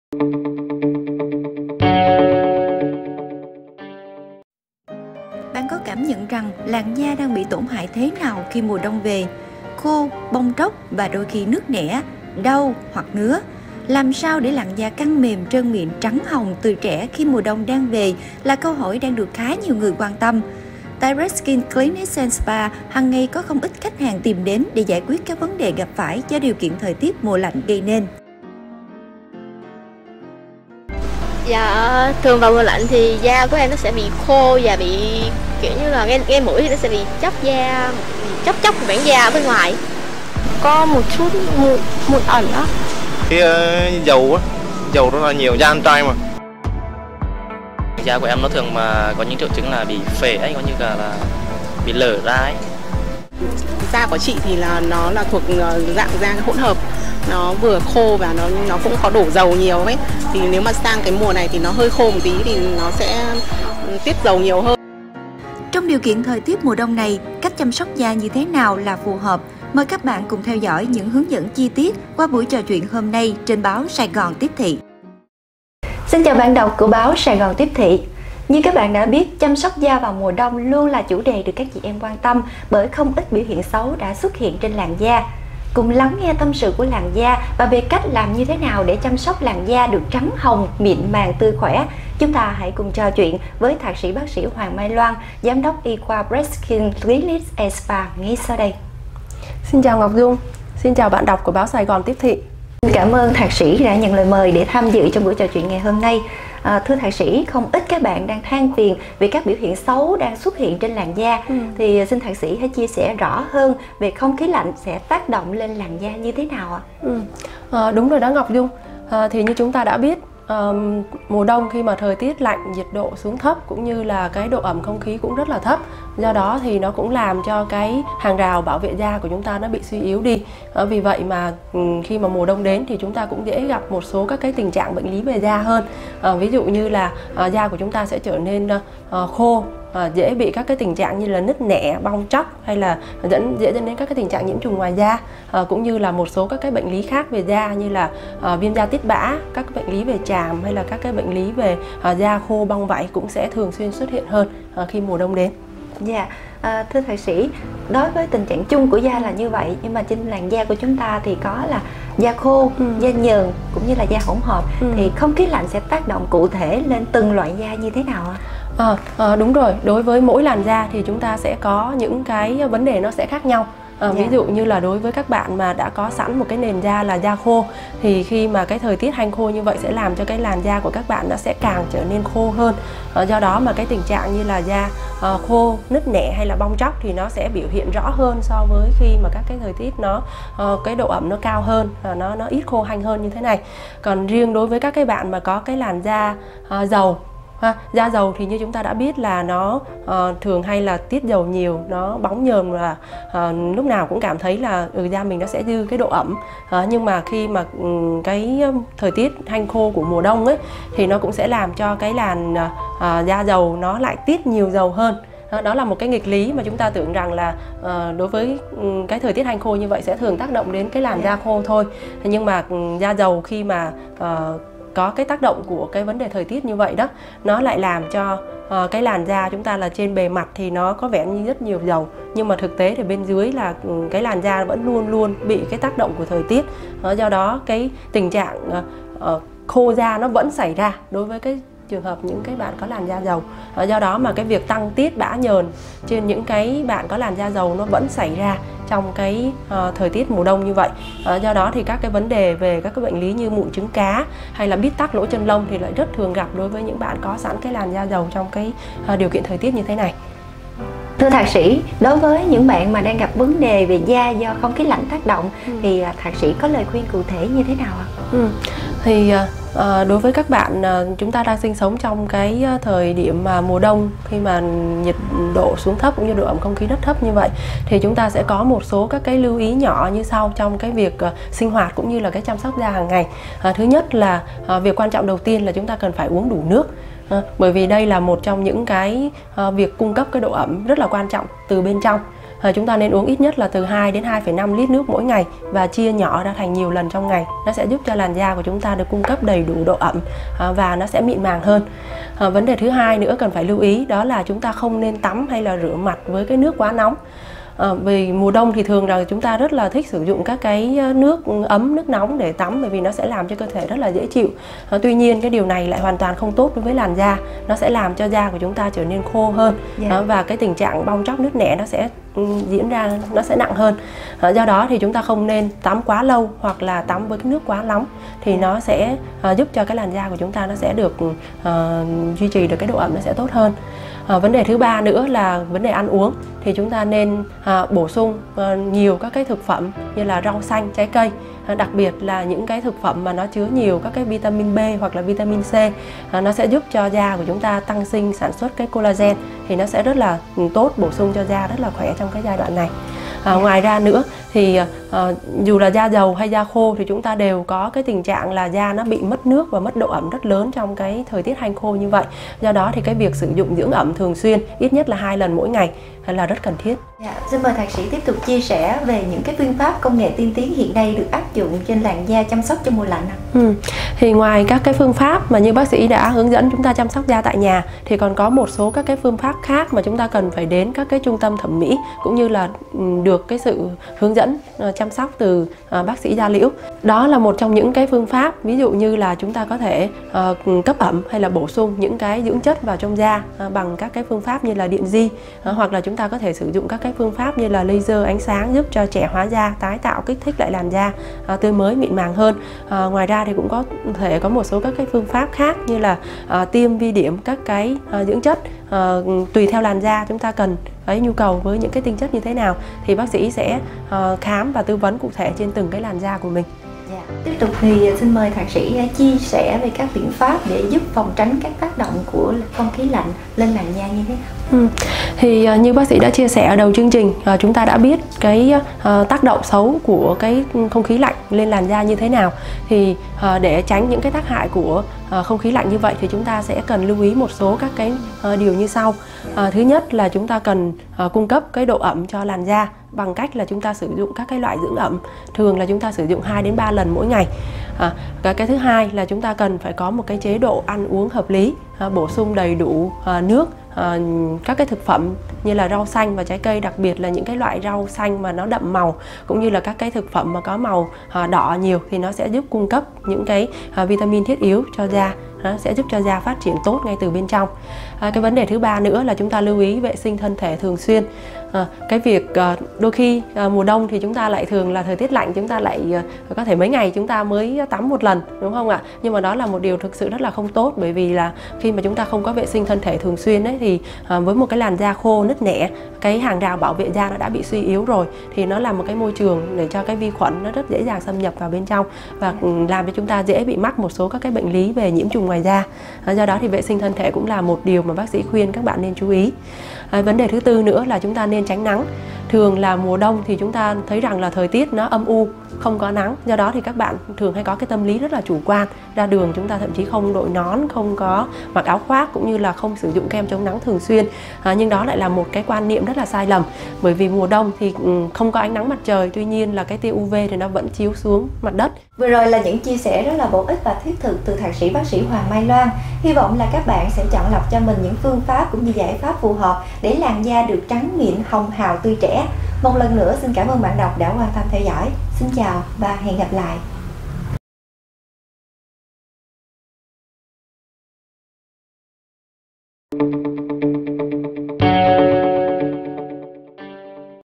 Những lưu ý từ bác sĩ để chăm sóc làn da khi trời chuyển lạnh
(SGTT) – Những ngày gần đây trời bắt đầu chuyển lạnh dẫn đến những phản ứng biểu hiện trên làn da. Có thật sự cần thiết phải tắm nước ấm, hạn chế rửa mặt, hay lười vệ sinh thân thể trong mùa lạnh ảnh hưởng ra sao đến làn da là câu hỏi đang khiến nhiều bạn đắn đo, suy nghĩ thời gian này.

Theo Thạc sĩ , bác sĩ Hoàng Mai Loan, Giám đốc y khoa FreshSkin Clinic & Spa (TPHCM), vào mùa lạnh, da dầu sẽ bị bóng nhờn nhiều hơn, còn da khô có thể tăng bong tróc nứt nẻ. Đối với mỗi loại da cần chế độ chăm sóc phù hợp, nếu cần thiết hãy đến trung tâm thẩm mỹ để được bác sĩ thăm khám và can thiệp công nghệ làm đẹp phù hợp hơn.

Mời bạn cùng tìm hiểu rõ hơn “tâm sự” của làn da trong mùa lạnh và lắng nghe những hướng dẫn chi tiết về cách chăm sóc phù hợp để làn da trở nên trắng hồng, mịn màng qua phần trao đổi cùng với Thạc sĩ, bác sĩ Hoàng Mai Loan trong video sau đây.